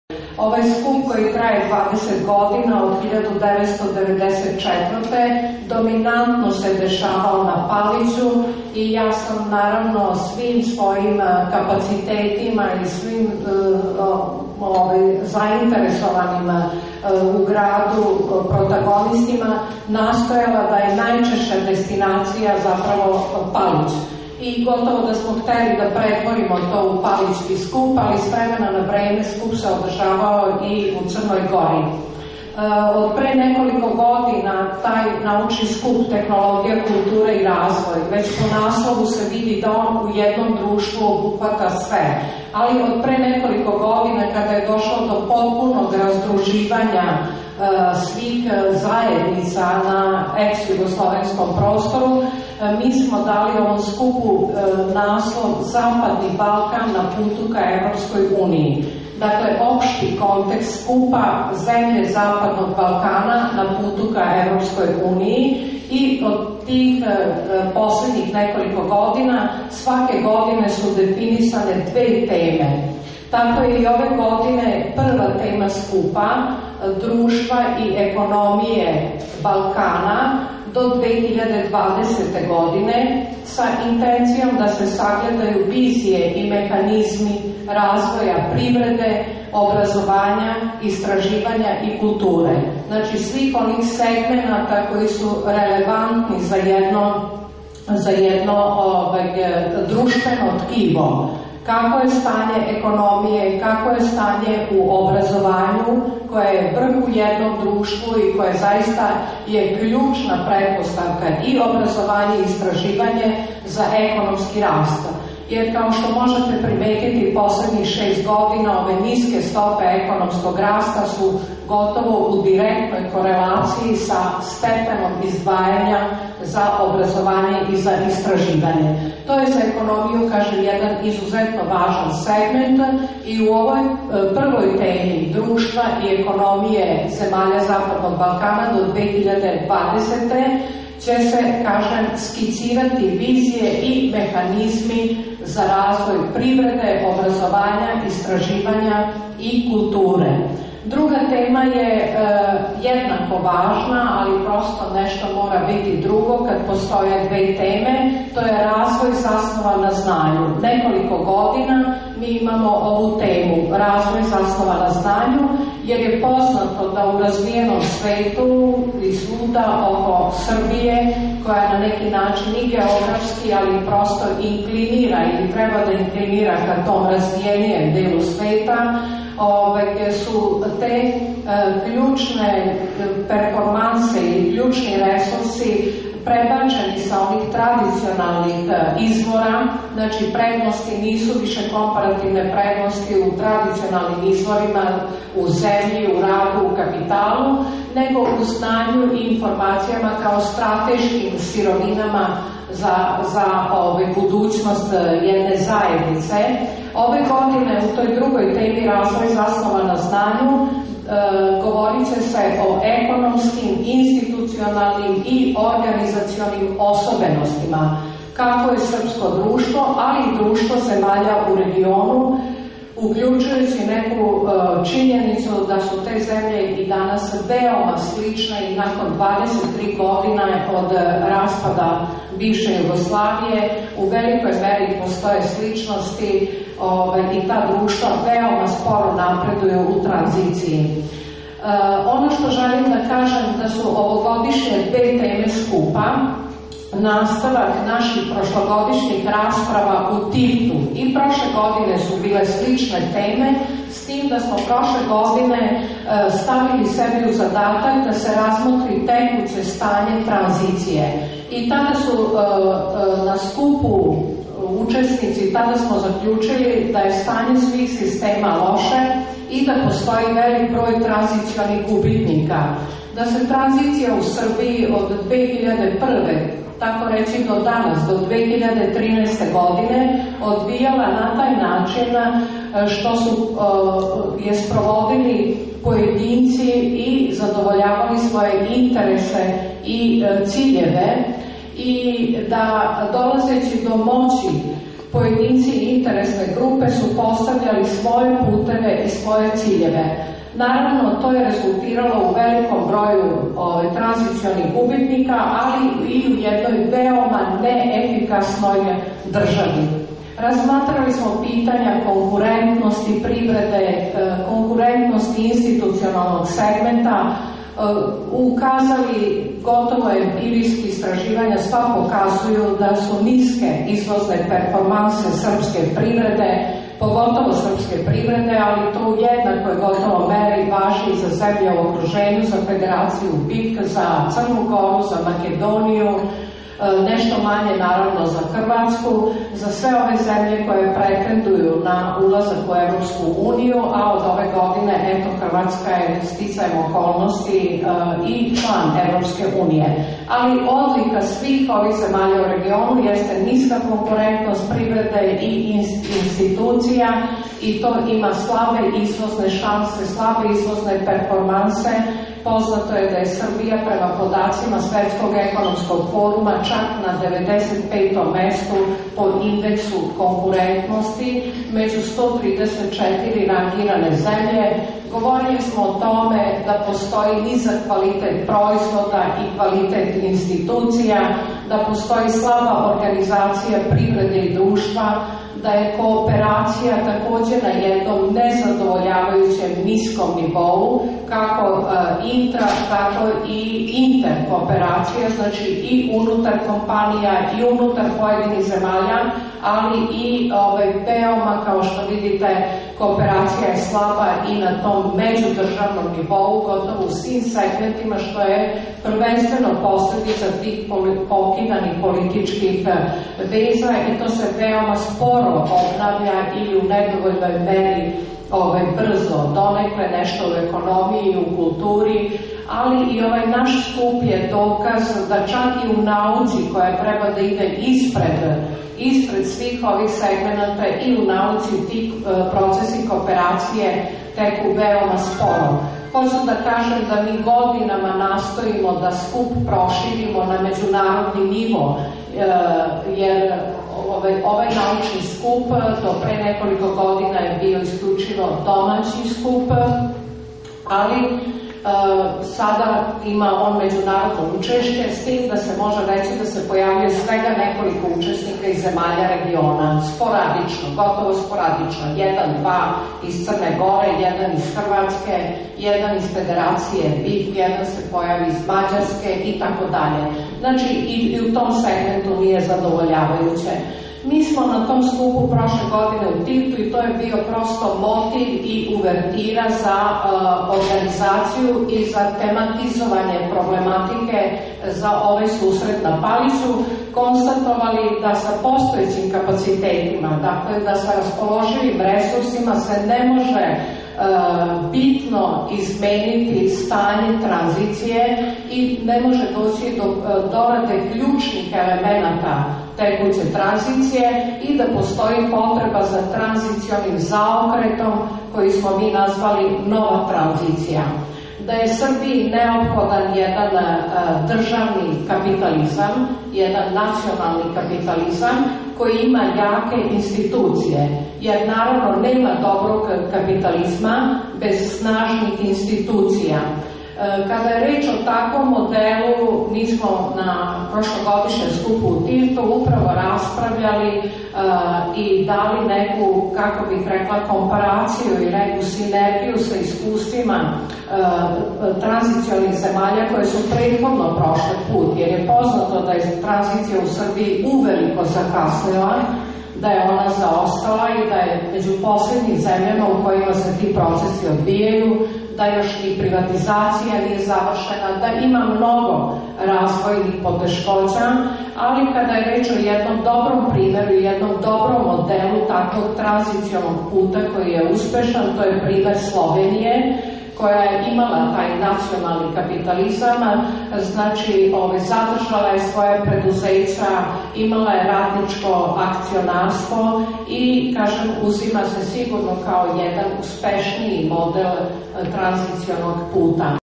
najava 20 naucnog skupa tehnologija kultura i razvoj
super-info.rs

20. Naučni skup "Tehnologija, kultura i razvoj"

Subotica, 27. avgusta - Jubilarni 20. naučni skup "Tehnologija, kultura i razvoj" održaće se od 3. do 5. septembra u Eko centru na Paliću, u sklopu obeležavanja proslave Dana grada.

Ovogodišnji jubilarni naučni skup okupiće 51 autora i koautora naučnih radova u okviru dve teme: "Društva i ekonomije Balkana 2020 - vizije i mehanizmi razvoja privrede, obrazovanja, istraživanja i kulture". Stalna tema kako u nekoliko prethodnih godina, tako i ovogodišnjeg skupa je "Razvoj zasnovan na znanju: ekonomske, organizacione i institucionalne osobenosti".
Predstavljanje naučnih radova i diskusije obuhvatiće problem nedovršene tranzicije i neophodnost da se izvrši tranzicija tranzicije u zemljama Zapadnog Balkana. Kako u Srbiji, tako i u zemljama regiona postoje gubitnici tranzicije, jer se ovaj proces odvijao tako što su tranziciju sprovodili pojedinci u skladu sa ličnim, uskim interesima i ciljevima, rekla je predsednica Organizacionog odbora prof. dr Danica Drakulić najavljujući ovogodišnji jubilarni naučni skup čija je ambicija da održi nivo međunarodnog naučnog skupa. Srbiji je neophodan jak nacionalni kapitalizam koji ima jake institucije, zaključak je sa prošlogodišnjeg 19. naučnog skupa održanog u Tivtu, što je model o kom će se diskutovati i na predstojećem naučnom skupu na Paliću, u okviru kog će biti upriličen i besplatan koncert za učesnike skupa i građane Subotice. Koncert će biti održan 4. septembra u Eko centru na Paliću od 19 časova.
Organizator skupa je Udruženje "Tehnologija i društvo", Ekonomski fakultet Subotica i Grad Subotica su glavni suorganizatori, a suorganizatori su Centar za istraživanje razvoja nauke i tehnologije - Institut "Mihajlo Pupin" iz Beograda i Elektrotehnički fakultet iz Podgorice.
Svečano otvaranje skupa zakazano je za 3. septembar u 10 časova u Eko centru na Paliću.